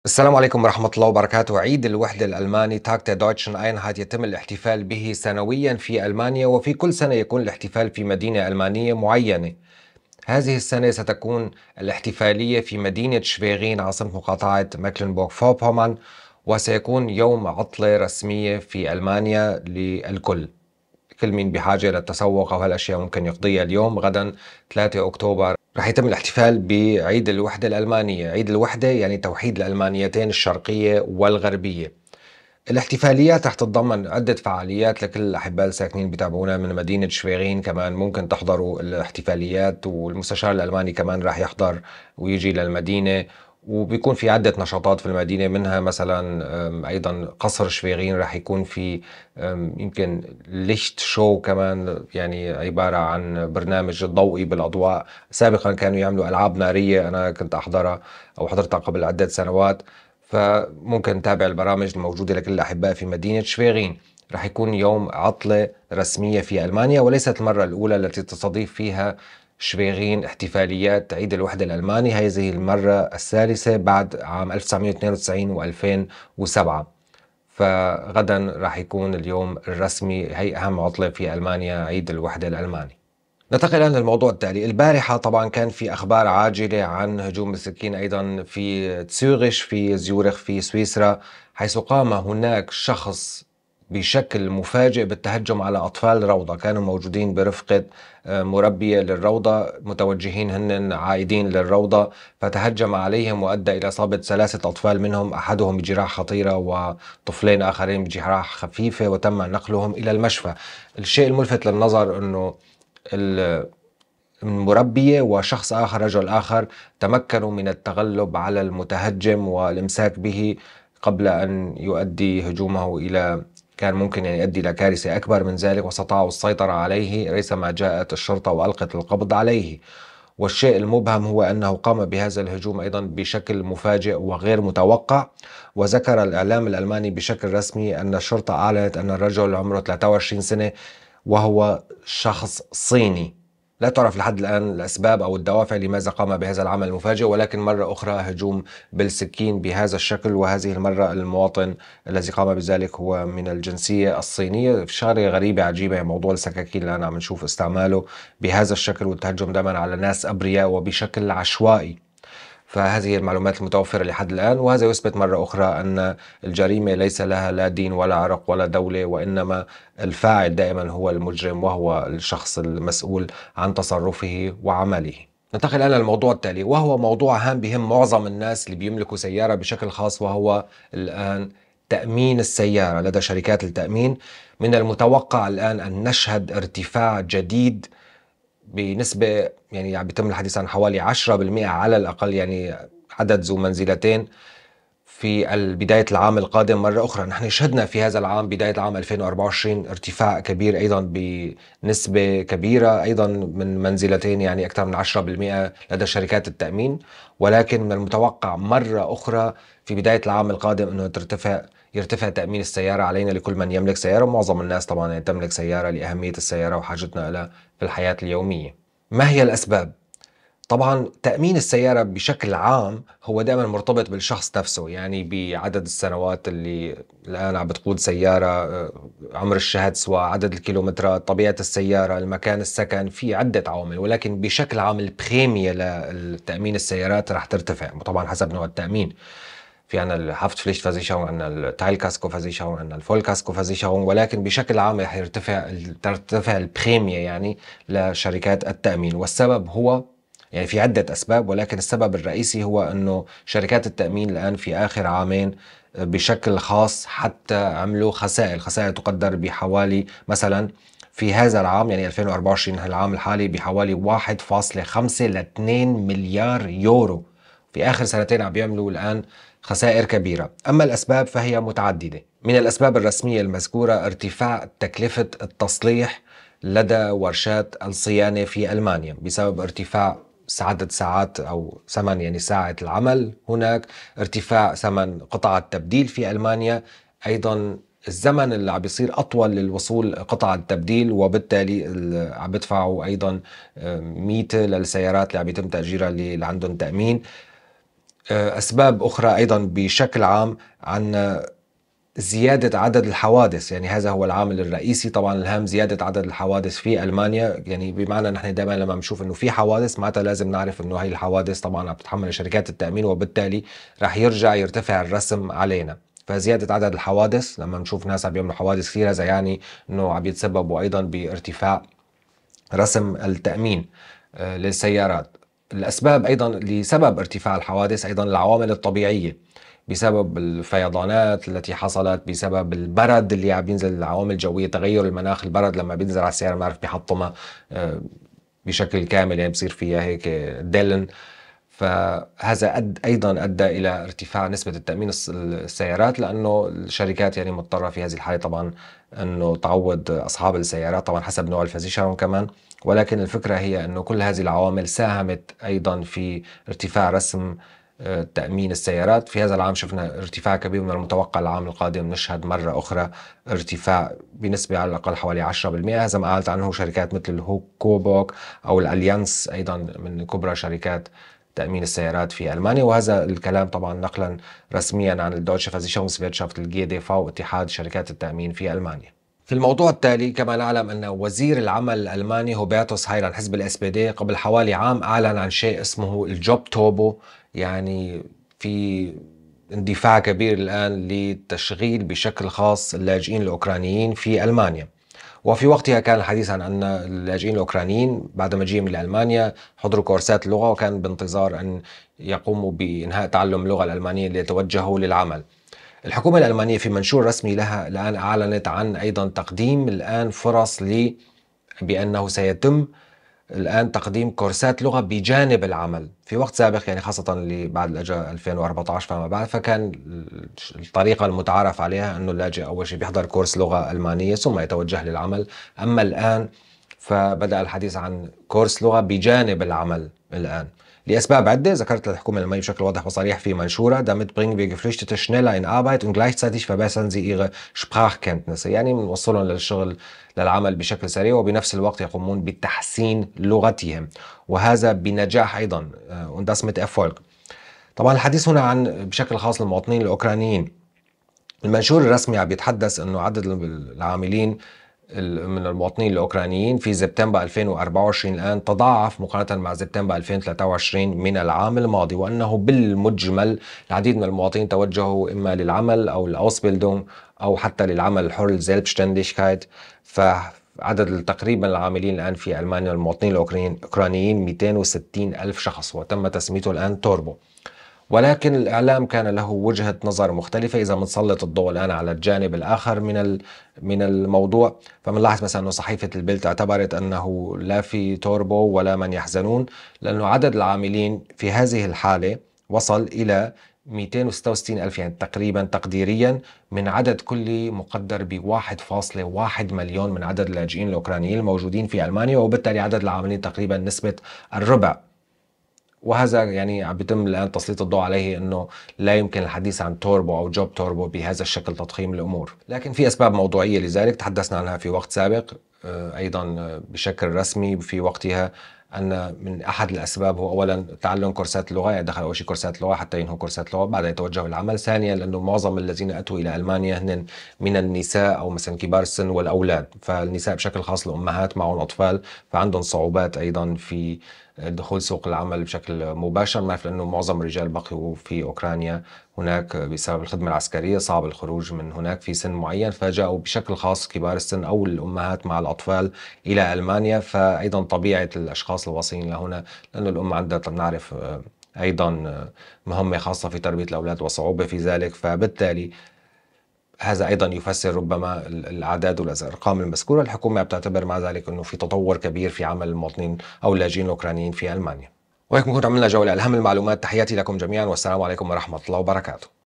أهم أخبار ألمانيا || ارتفاع كبير متوقع لرسم تأمين السيارات في ألمانيا
Timestamps:
►00:00 عيد الوحدة الألماني
►01:23 هجوم في زيورخ السويسرية
►02:55 ارتفاع كبير متوقع لرسم تأمين السيارات في ألماني
►06:20 تشغيل اللاجئين بجانب كورس اللغة

▬ Khaled Life القناة الثانية  ▬▬▬▬▬▬▬▬▬▬▬▬▬▬▬
معكم الرحلات والسفر و المغامرات أحلى. رافقوني في رحلاتي للتعرف على البلدان و الناس و الثقافات  و الحضارات المتنوعة